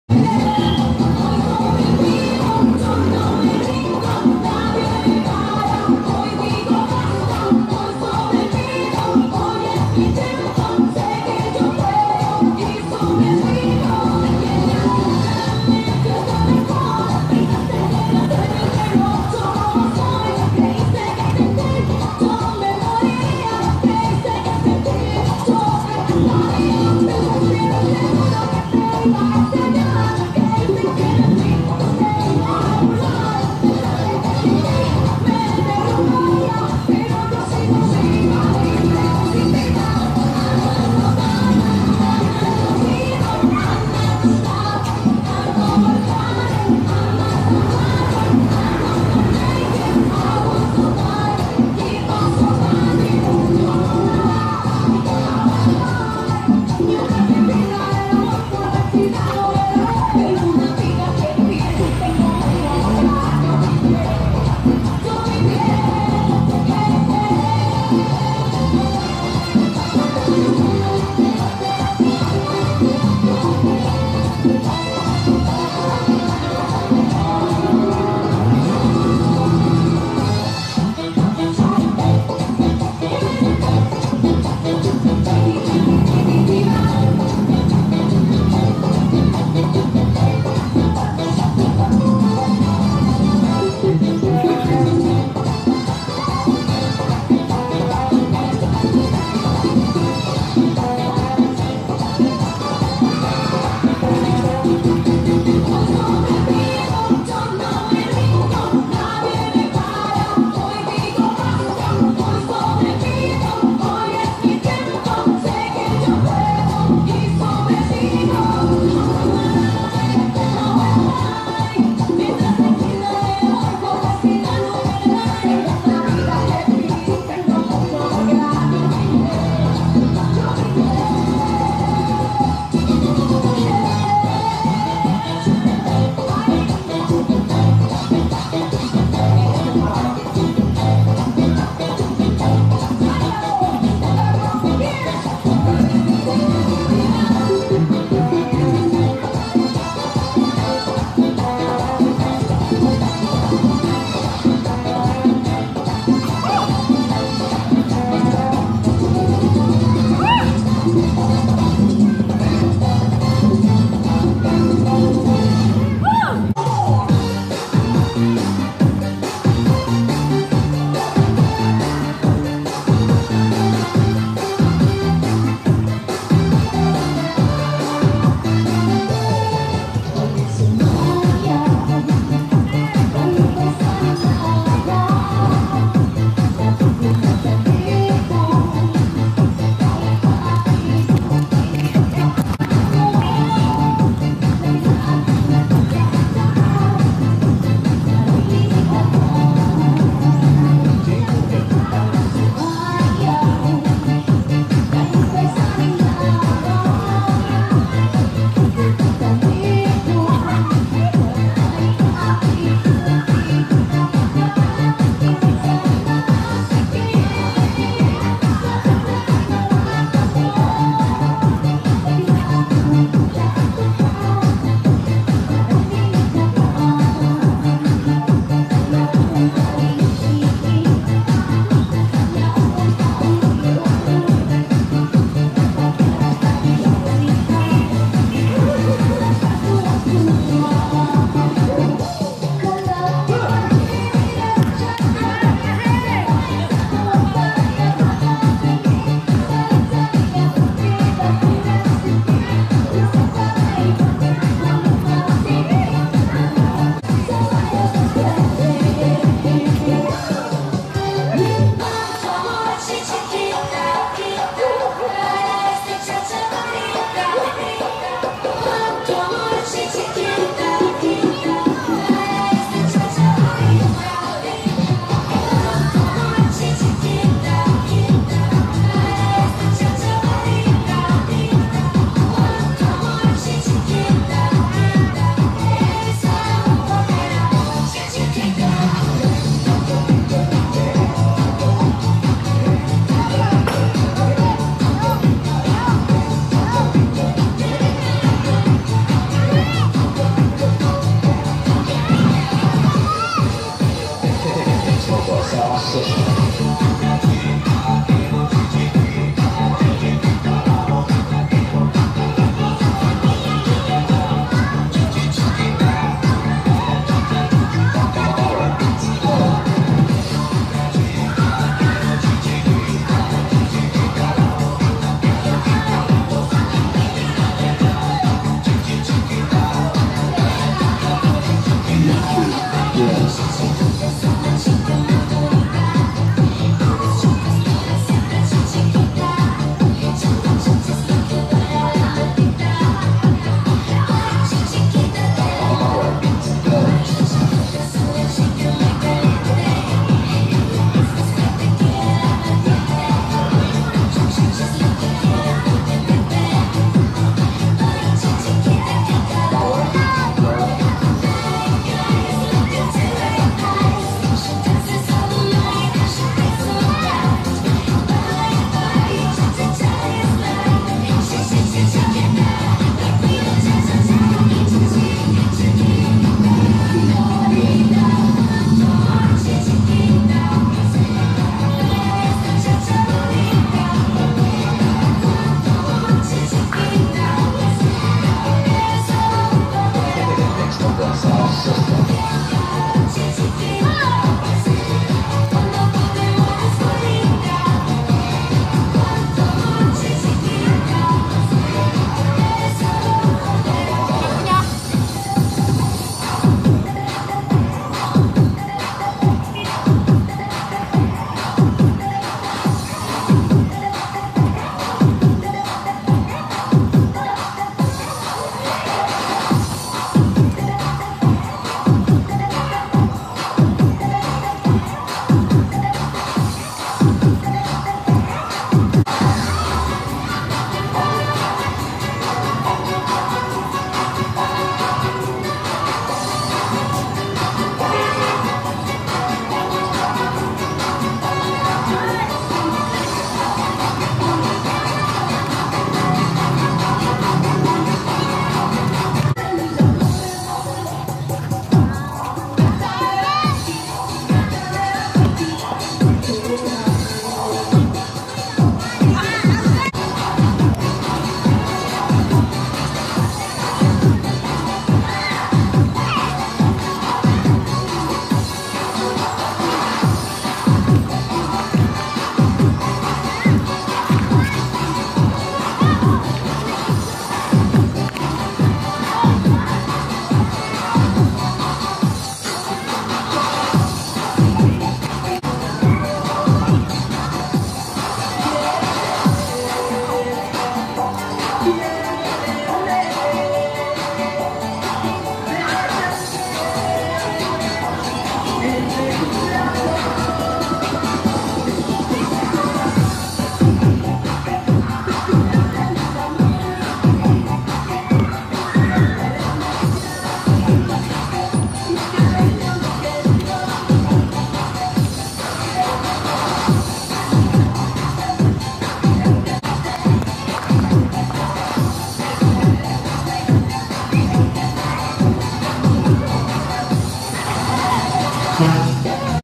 zumba zumbafitness zumbadance freedance dance dancelover exercise dancing friend relaxing
free to join our Zumba here in Alcoy Cebu.  You can meet friendly people and If you do not know how to dance it is OK because you can dance your own steps.